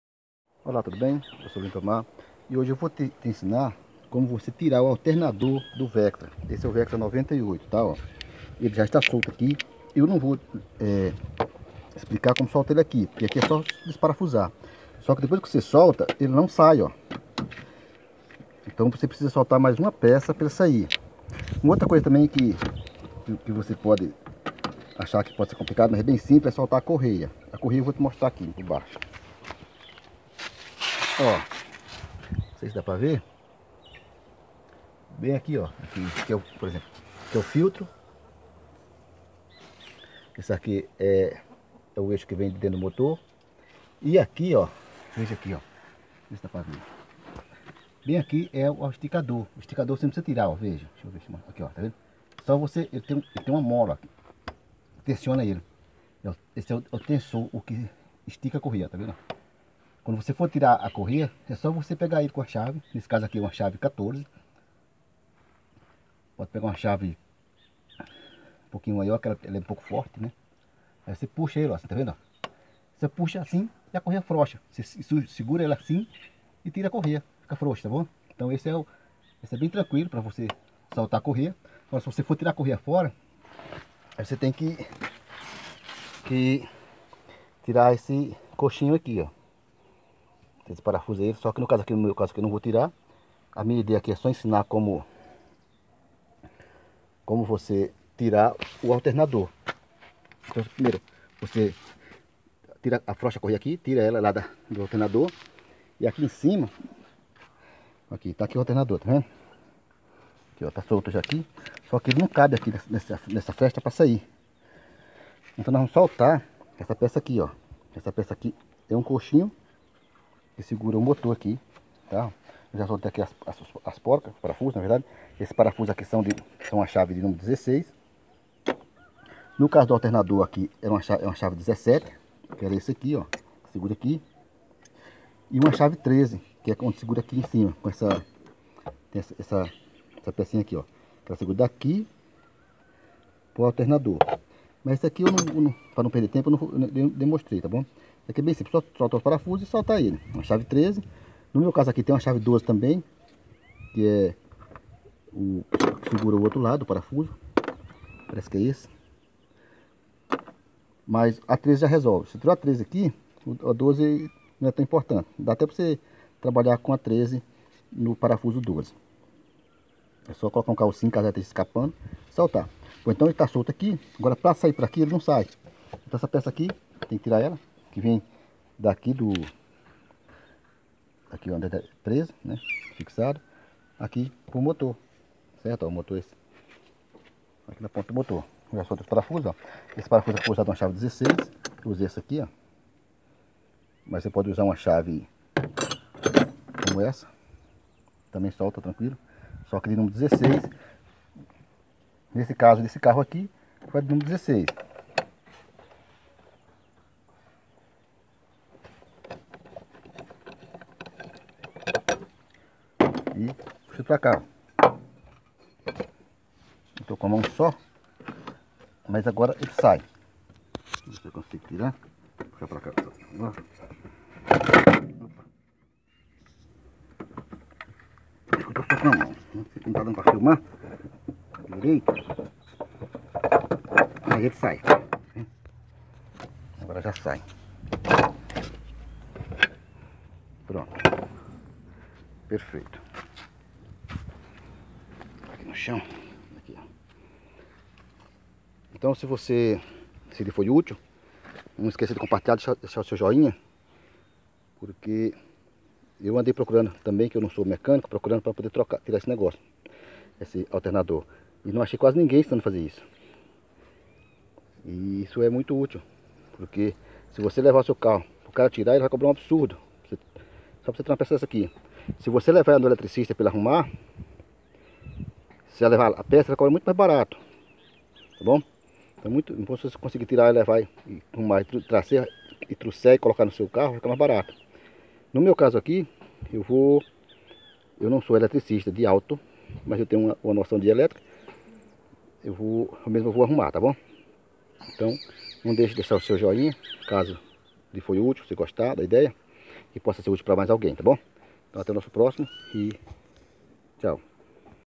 como tirar o alternador do vectra 98
como tirar o alternador do vectra 98 nesse vídeo mostro passo a passo como tirao o auternador do vectra, e tambem mostro comom é simples soltar e tirar a correia do alternador do vectra